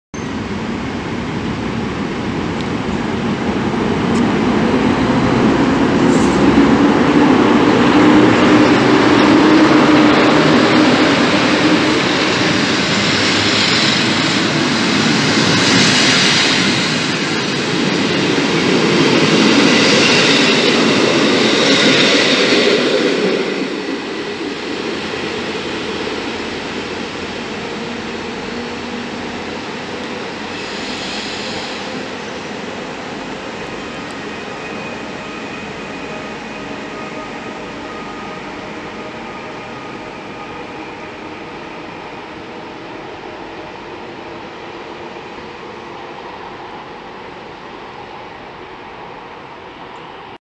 Trapez z Oscypkiem |Sosnowiec Główny
Szybki odjazd EP07-338 z IC Oscypek do Warszawy ze stacji Sosnowiec Główny.

Pozdrawiam i zapraszam na film! :)